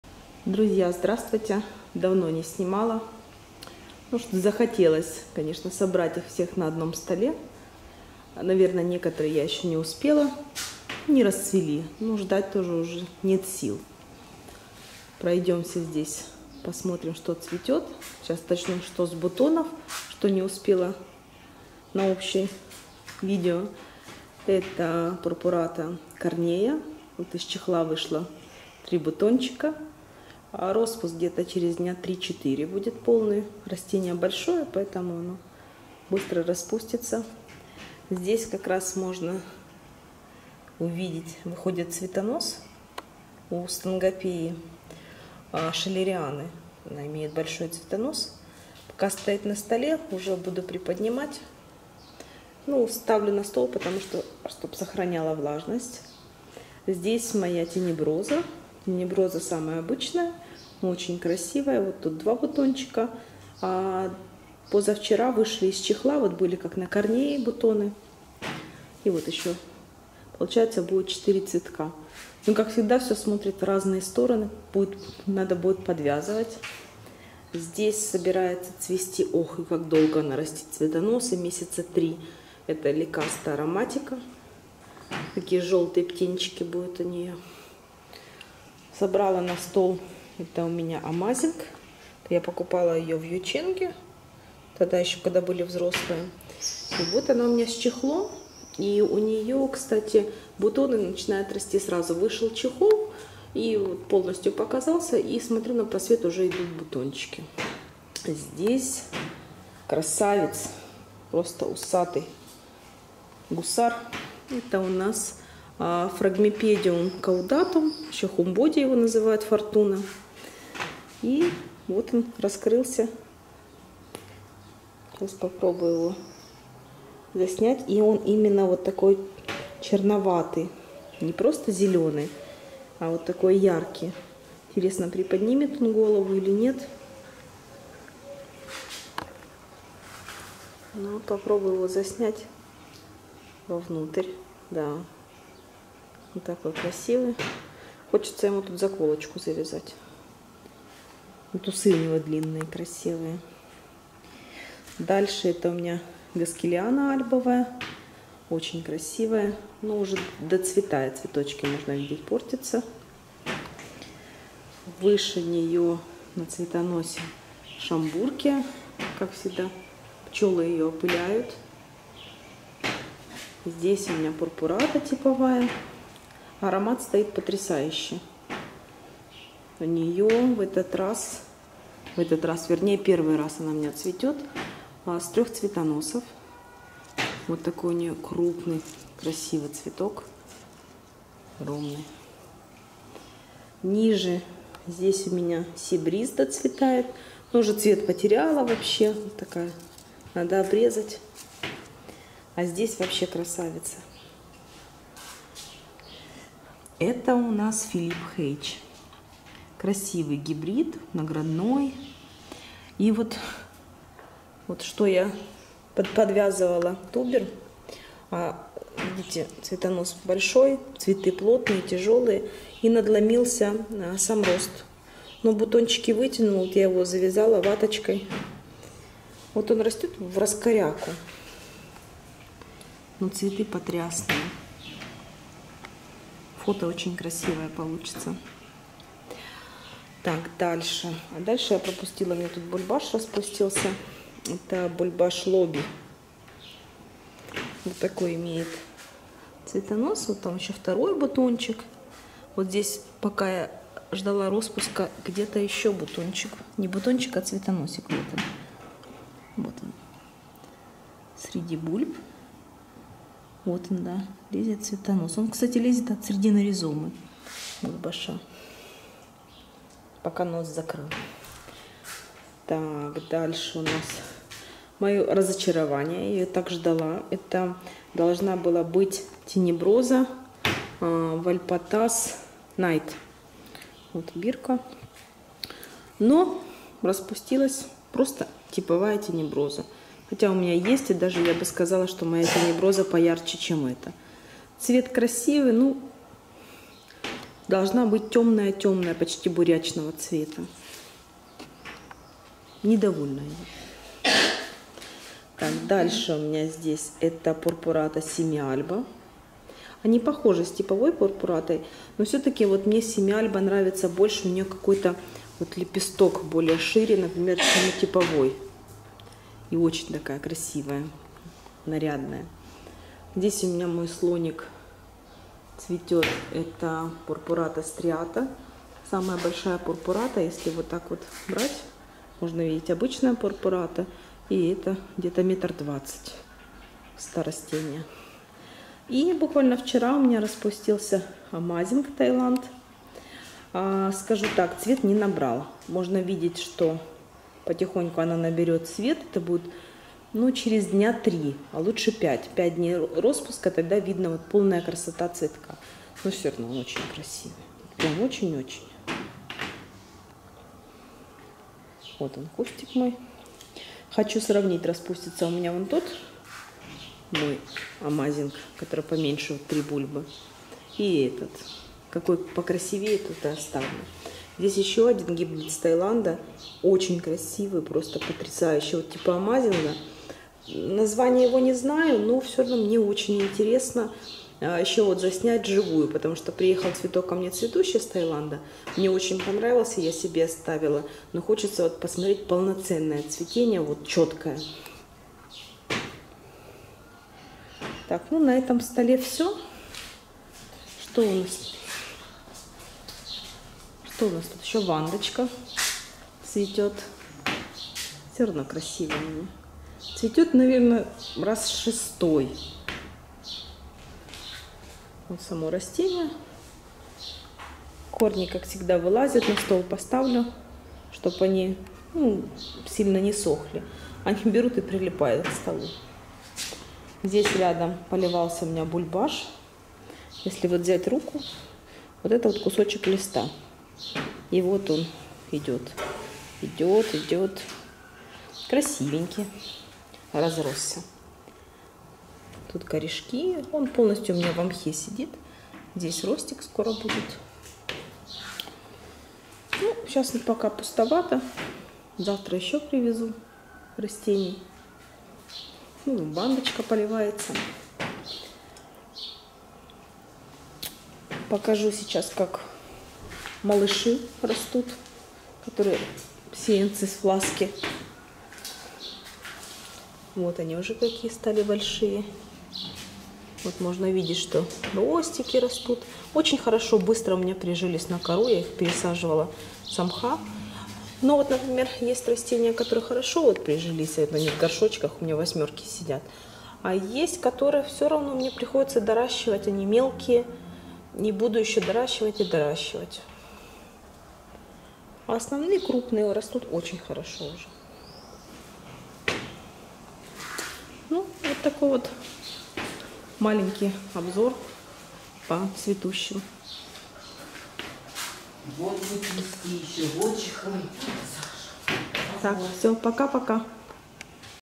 Орхидеи в теплице цветут. ИЮНЬ
На видео снято цветение :Laelia purpurata semi-alba, var striata  ,var. tipo, Schomburgkia tibicinis,  Phragmipedium caudatum 'Fortuna', Blc. Felipe Haje, Blc. Felipe Haje, Rlc.Amazing Thailand  , Vanda Udom Gold x Ascda Suk Sumrat Spot